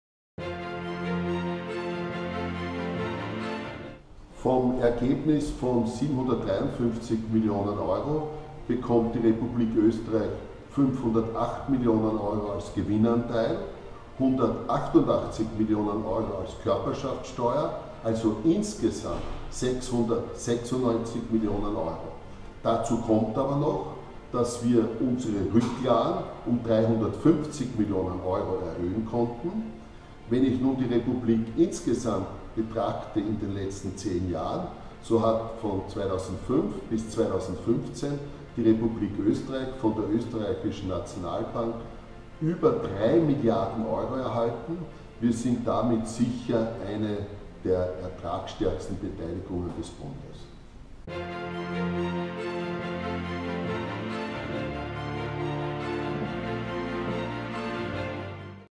OeNB Geschäftsbericht 2015 – Gewinnabfuhr an die Republik Österreich
Die Republik erhält vom geschäftlichen Ergebnis 696 Mio EUR (2014: 315 Mio EUR), wovon 188 Mio EUR auf die Körperschaftsteuer und 508 Mio EUR auf den 90%igen Gewinnanteil entfallen. Präsident Dr. Claus J. Raidl erläutert die Ergebnisse der Bilanz Struktur für das Jahr 2015.